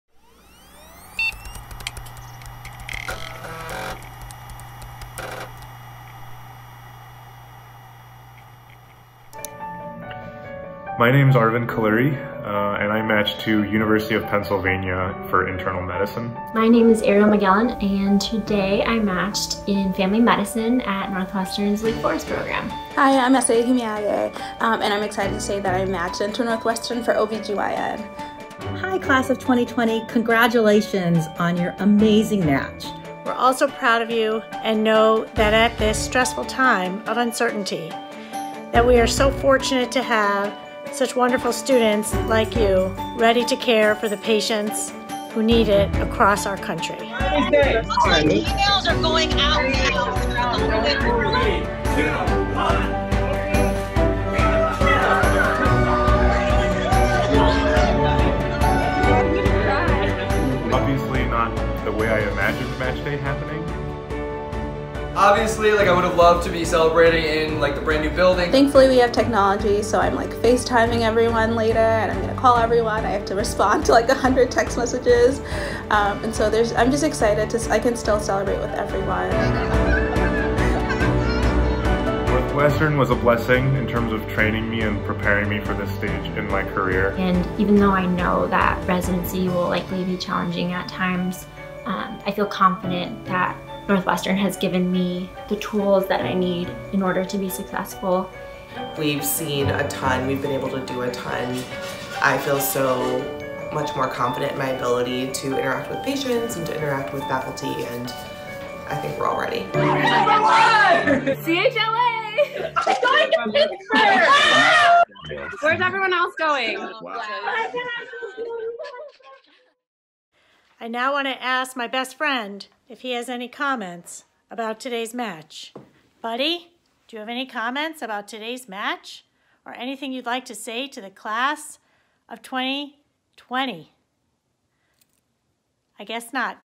Match Day 2020 Northwestern
On Match Day, an annual tradition held on the third Friday of March, all fourth-year medical students across the country learn at the same time where they will train as residents for the next three to seven years. This year, Northwestern students learned their match online and celebrated virtually, following public health measures restricting public gatherings, aimed at slowing the spread of the COVID-19 pandemic.